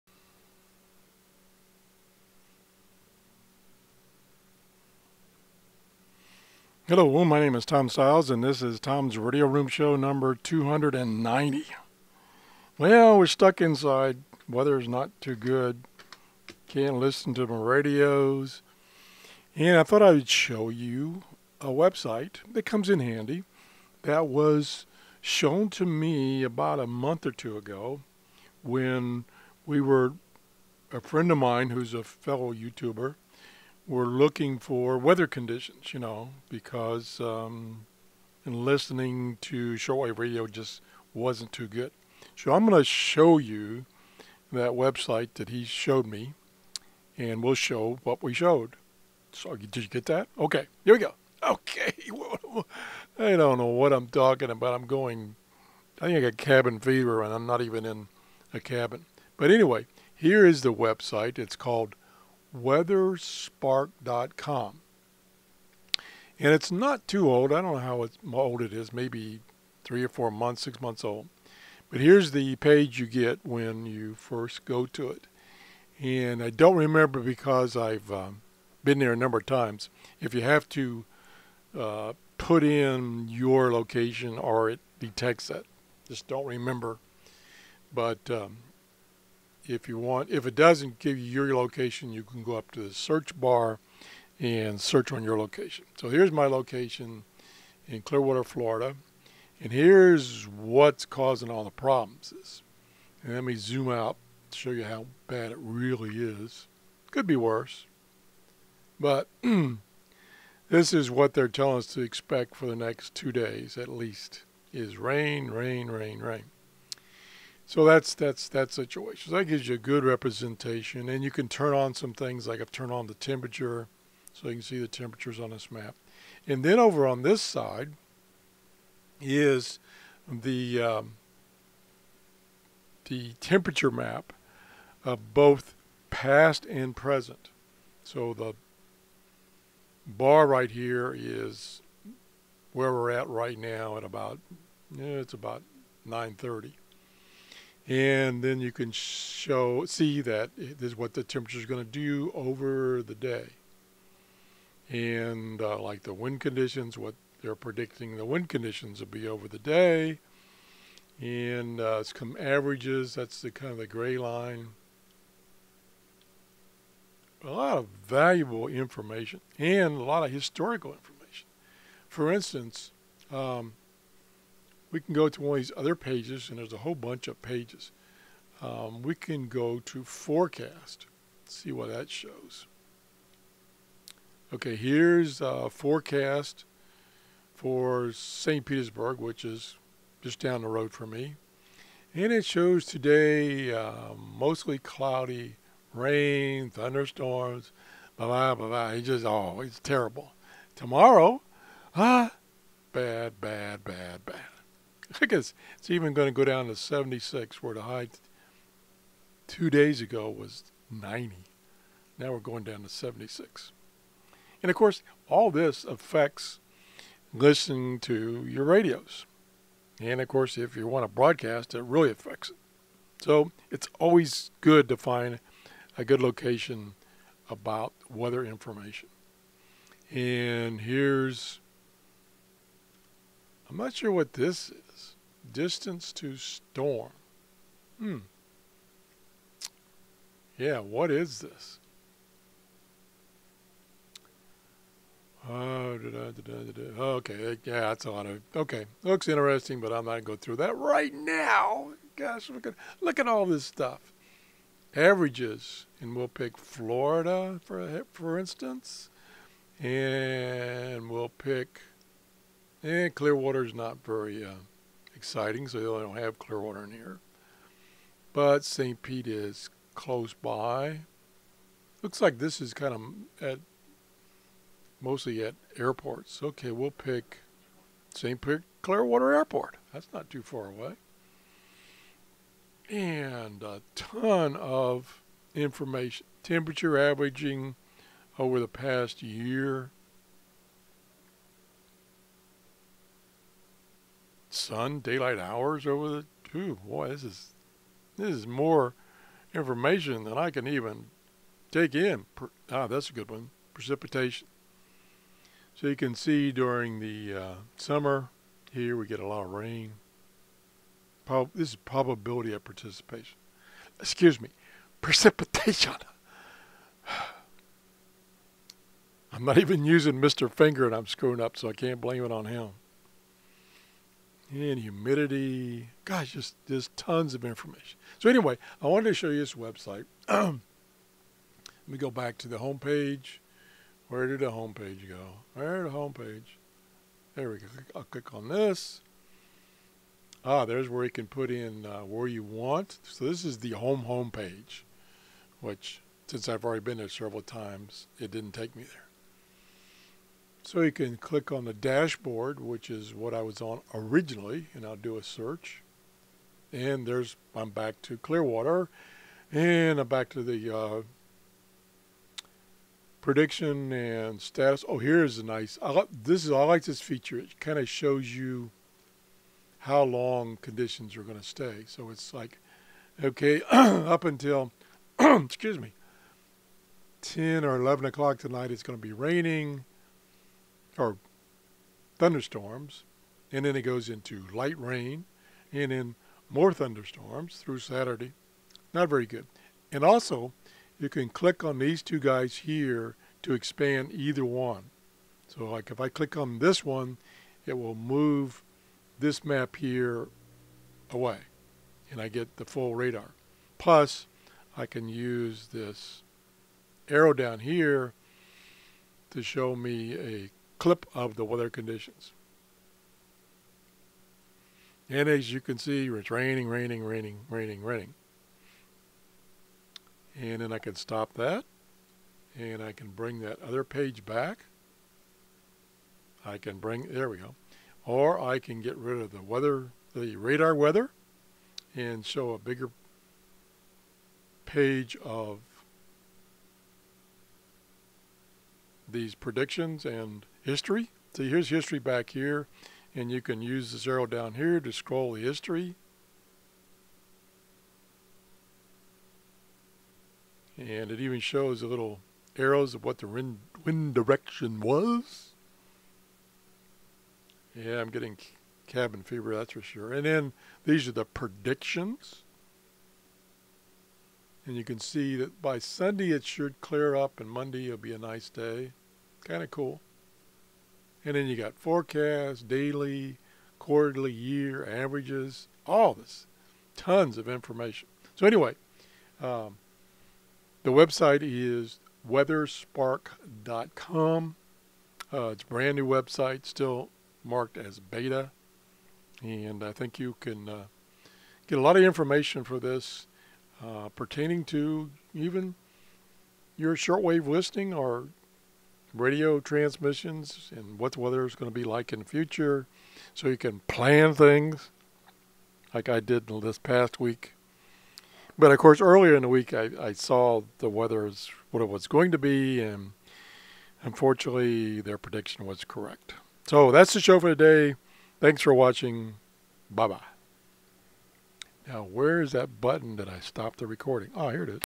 TRRS #0290 - Shortwave Listening Poor? Check the Weather Map
If you are having problems listening to shortwave radio, maybe it's the weather conditions.  Here is a great website showing you a lot of information on the weather conditions past, present and future.